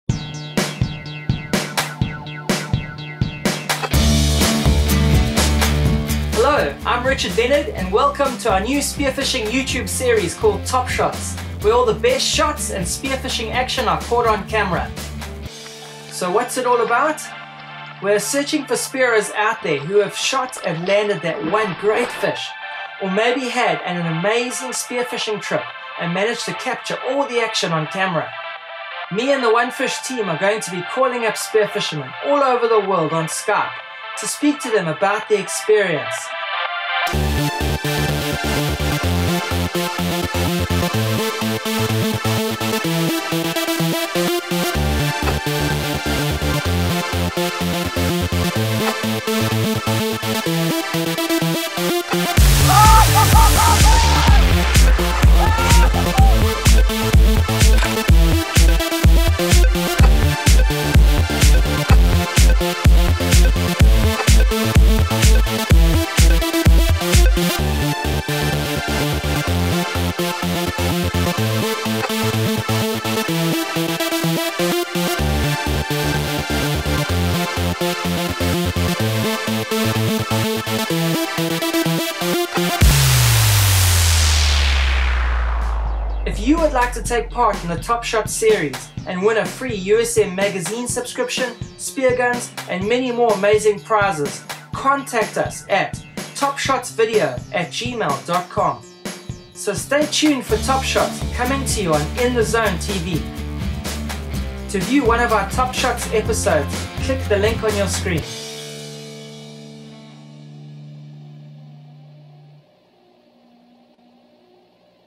Top Shots - The Best Spearfishing Shots Series
Welcome to the 'Top Shots' Spearfishing Video Series.
This is a fantastic new video concept by In The Zone where we will be searching the globe for the guys with the most insane spearfishing footage on the most incredible fish.
Each amazing spearfishing episode will feature the 'Top Shots' pick of the month based on the quality of the video, how the shot is captured and how impressive the fish is.

We will then call up the winner and get their unique side ot their story. We want to find out how it all went down and find out a little more about the diver. The guys who get chosen to feature on the 'Top Shots' series will win a magazine subscription from the Ultimate Spearfishing Magazine & much, much more ..

Directed By:
Richard Leonard

Edited By:
Matthew Swart

Filmed By:
Richard Leonard
Chris Coates
Robin Moulang
Mohammed Jassim Al Kuwar
Mubarak Rashed Al-Hajri